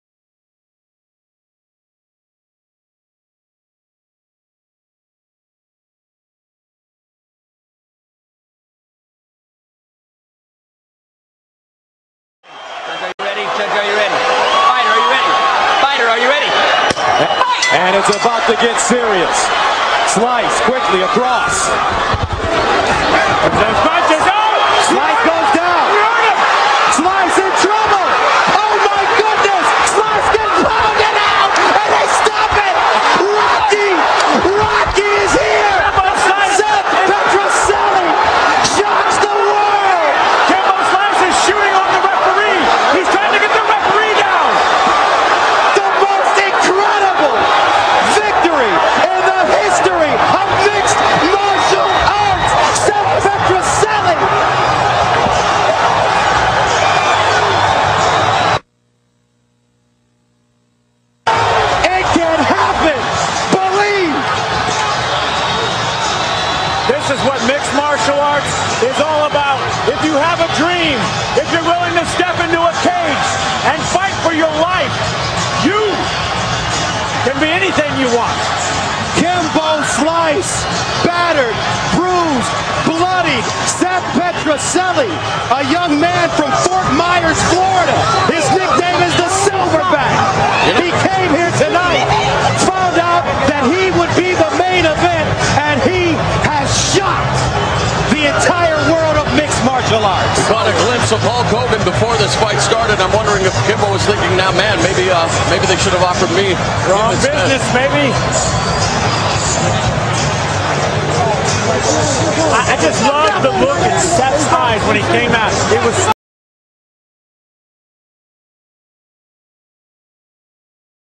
Street Fights - Kimbo Slice Knocked Out!
in this UFC MMA Fighting video, Kimbo Slice loses to seth Petruzelli .